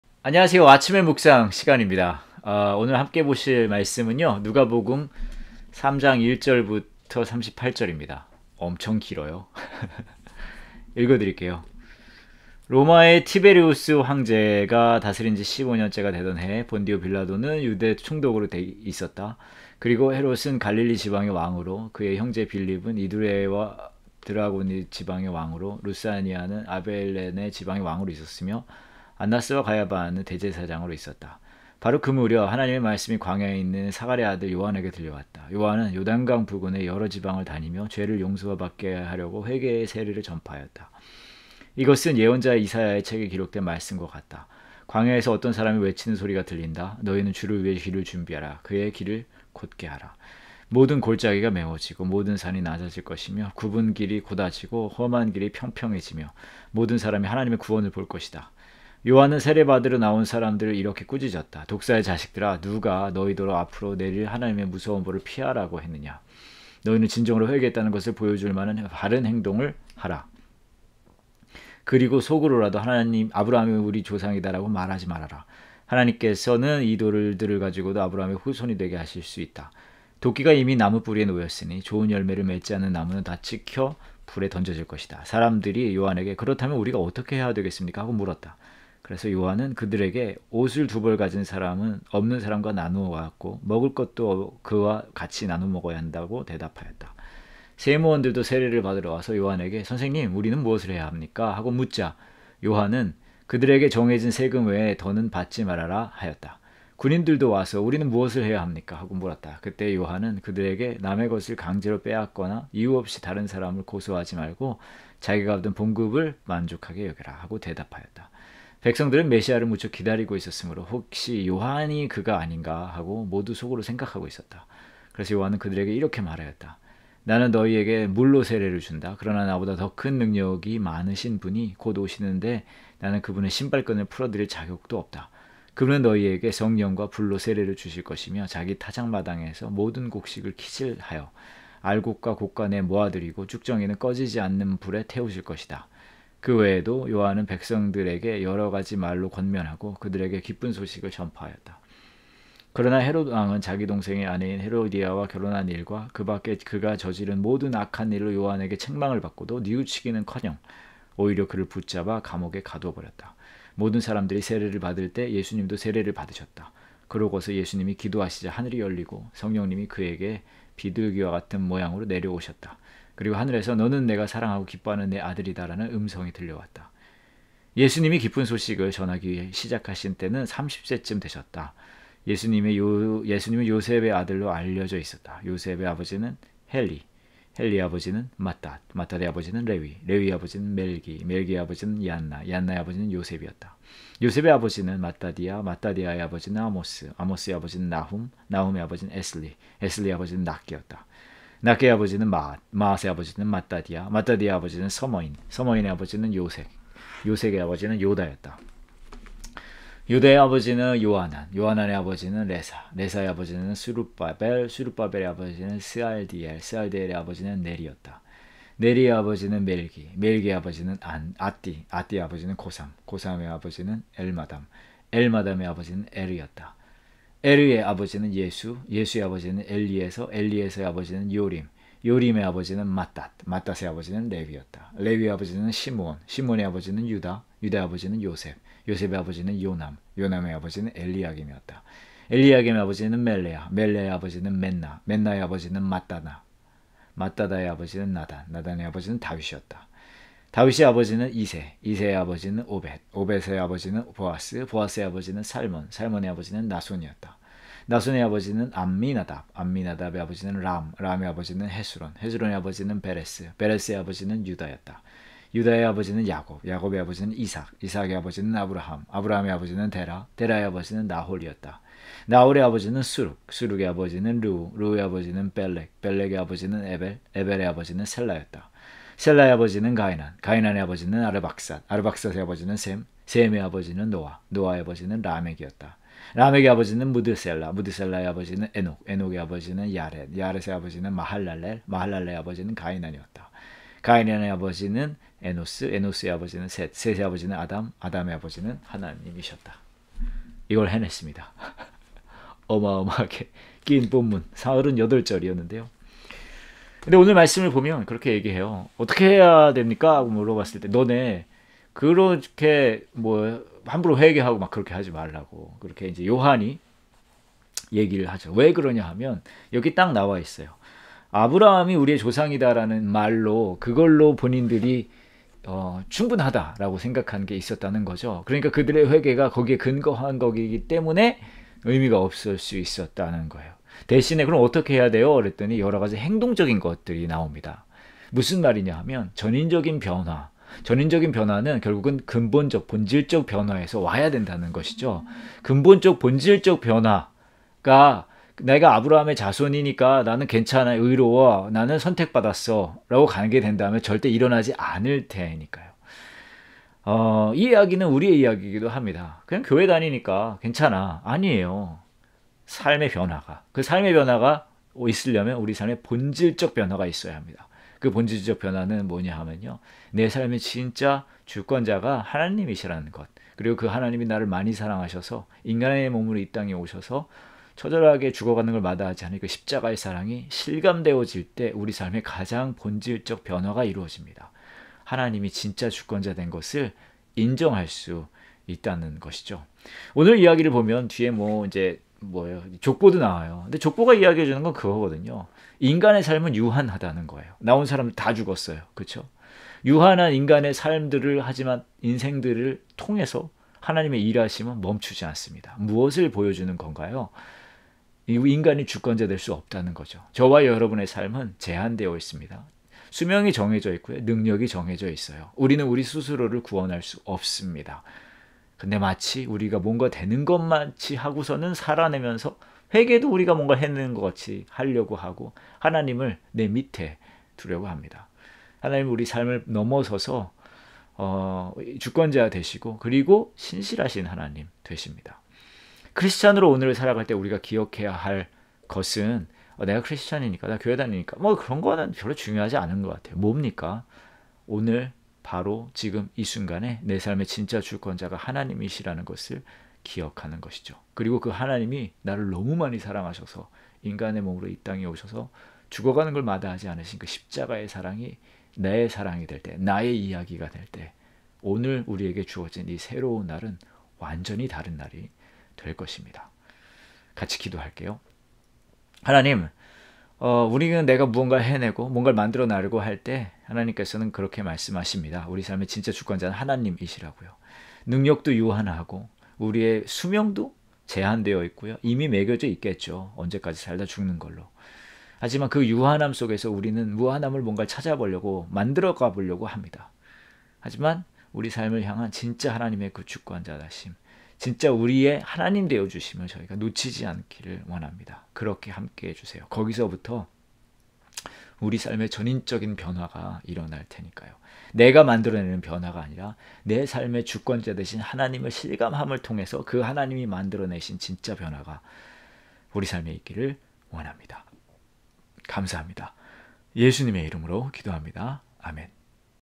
11/8(수) [아침N묵상] | 와플터치QT | 누가복음 3:1-38 (써어니)
3:1 로마의 티베리우스황제가 다스린 지 15년째가 되던 해에 본디오 빌라도는 유대 총독으로 있었다. 그리고 헤롯은 갈릴리 지방의 왕으로, 그의 형제 빌립은 이두래와 드라고닛 지방의 왕으로, 루사니아는 아빌레네 지방의 왕으로 있었으며

3:2 안나스와 가야바는 대제사장으로 있었다. 바로 그 무렵 하나님의 말씀이 광야에 있는 사가랴의 아들 요한에게 들려왔다.

3:3 요한은 요단강 부근의 여러 지방을 다니며 죄를 용서받게 하려고 회개의 세례를 전파하였다.

3:4 이것은 예언자 이사야의 책에 기록된 말씀과 같다. : '광야에서 어떤 사람의 외치는 소리가 들린다. 너희는 주를 위해 길을 준비하라. 그의 길을 곧게 하라.

3:5 모든 골짜기가 메워지고 모든 산이 낮아질 것이며 굽은 길이 곧아지고 험한 길이 평평해지며

3:6 모든 사람이 하나님의 구원을 볼 것이다.'

3:7 요한은 세례받으러 나오는 사람들을 이렇게 꾸짖었다. '독사의 자식들아 ! 누가 너희더러 앞으로 내릴 하나님의 무서운 벌을 피하라고 했느냐 ?

3:8 너희는 진정으로 회개했다는 것을 보여 줄 만한 바른 행동을 하라. 그리고 속으로나마 '아브라함이 우리 조상이다.'라고 말하지 말아라. 하나님께서는 이 돌들을 가지고도 아브라함의 후손이 되게 하실 수 있다.

3:9 도끼가 이미 나무 뿌리에 놓였으니 좋은 열매를 맺지 않는 나무는 다 찍혀 불에 던져질 것이다.'

3:10 사람들이 요한에게 '그렇다면 우리가 어떻게 해야 되겠습니까 ?' 하고 물었다.

3:11 그래서 요한은 그들에게 '옷을 두 벌 가진 사람은 없는 사람과 나누어 갖고 먹을 것도 그와 같이 나누어 먹어야 한다.' 하고 대답하였다.

3:12 세무원들도 세례를 받으러 와서 요한에게 '선생님, 우리는 무엇을 해야 합니까 ?' 하고 묻자

3:13 요한은 그들에게 '정해진 세금 외에는 더 받지 말아라.' 하였다.

3:14 군인들도 와서 '우리는 무엇을 해야 합니까 ?' 하고 물었다. 그때 요한은 그들에게 '남의 것을 강제로 빼앗거나 이유 없이 다른 사람을 고소하지 말고 자기가 받는 봉급을 만족하게 여겨라.' 하고 대답하였다.

3:15 백성들은 메시야를 무척 기다리고 있었으므로 혹시 요한이 그가 아닌가 하고 모두 속으로 생각하고 있었다.

3:16 그래서 요한은 그들에게 이렇게 말하였다. '나는 너희에게 물로 세례를 준다. 그러나 나보다 더 능력이 많으신 분이 곧 오시는데 나는 그분의 신발끈을 풀어 드릴 자격도 없다. 그분은 너희에게 성령과 불로 세례를 주실 것이며

3:17 자기 타작 마당에서 모든 곡식을 키질하여 알곡은 곳간에 모아 들이고 쭉정이는 꺼지지 않는 불에 태우실 것이다.'

3:18 그 외에도 요한은 백성들에게 여러 가지 말로 권면하고 그들에게 기쁜 소식을 전파하였다.

3:19 그러나 헤롯왕은 자기 동생의 아내인 헤로디아와 결혼한 일과 그 밖에 그가 저지른 모든 악한 일로 요한에게 책망을 받고도

3:20 뉘우치기는커녕 오히려 그를 붙잡아 감옥에 가둬 버렸다.

3:21 모든 사람들이 세례를 받을 때 예수님도 세례를 받으셨다. 그리고서 예수님이 기도하시자 하늘이 열리고

3:22 성령님이 그에게 비둘기와 같은 모양으로 내려오셨다. 그리고 하늘에서 '너는 내가 사랑하고 기뻐하는 내 아들이다.'라는 음성이 들려왔다.

3:23 예수님이 기쁜 소식을 전하기 시작하신 때는 30세쯤 되셨다. 예수님은 요셉의 아들로 알려져 있었다. 요셉의 아버지는 헬리,

3:24 헬리의 아버지는 맛닷, 맛닷의 아버지는 레위, 레위의 아버지는 멜기, 멜기의 아버지는 얀나, 얀나의 아버지는 요셉이었다.

3:25 요셉의 아버지는 맛다디아, 맛다디아의 아버지는 아모스, 아모스의 아버지는 나훔, 나훔의 아버지는 에슬리, 에슬리의 아버지는 낙개였다.

3:26 낙개의 아버지는 마앗, 마앗의 아버지는 맛다디아, 맛다디아의 아버지는 서머인, 서머인의 아버지는 요섹, 요섹의 아버지는 요다였다.

3:27 요다의 아버지는 요아난, 요아난의 아버지는 레사, 레사의 아버지는 스룹바벨, 스룹바벨의 아버지는 스알디엘, 스알디엘의 아버지는 네리였다.

3:28 네리의 아버지는 멜기, 멜기의 아버지는 앗디, 앗디의 아버지는 고삼, 고삼의 아버지는 엘마담, 엘마담의 아버지는 에르였다.

3:29 에르의 아버지는 예수, 예수의 아버지는 엘리에서, 엘리에서의 아버지는 요림, 요림의 아버지는 맛닷, 맛닷의 아버지는 레위였다.

3:30 레위의 아버지는 시므온, 시므온의 아버지는 유다, 유다의 아버지는 요셉, 요셉의 아버지는 요남, 요남의 아버지는 엘리아김이었다.

3:31 엘리아김의 아버지는 멜레아, 멜레아의 아버지는 멘나, 멘나의 아버지는 맛다다, 맛다다의 아버지는 나단, 나단의 아버지는 다윗이었다.

3:32 다윗의 아버지는 이새, 이새의 아버지는 오벳, 오벳의 아버지는 보아스, 보아스의 아버지는 살몬, 살몬의 아버지는 나손이었다.

3:33 나손의 아버지는 암미나답, 암미나답의 아버지는 람, 람의 아버지는 헤스론, 헤스론의 아버지는 베레스, 베레스의 아버지는 유다였다.

3:34 유다의 아버지는 야곱, 야곱의 아버지는 이삭, 이삭의 아버지는 아브라함, 아브라함의 아버지는 데라, 데라의 아버지는 나홀이었다.

3:35 나홀의 아버지는 스룩, 스룩의 아버지는 르우, 르우의 아버지는 벨렉, 벨렉의 아버지는 에벨, 에벨의 아버지는 셀라였다.

3:36 셀라의 아버지는 가이난, 가이난의 아버지는 아르박삿, 아르박삿의 아버지는 셈, 셈의 아버지는 노아, 노아의 아버지는 라멕이었다.

3:37 라멕의 아버지는 므두셀라, 므두셀라의 아버지는 에녹, 에녹의 아버지는 야렛, 야렛의 아버지는 마할랄렐, 마할랄렐의 아버지는 가이난이었다.

3:38 가이난의 아버지는 에노스, 에노스의 아버지는 셋, 셋의 아버지는 아담, 아담의 아버지는 하나님이셨다.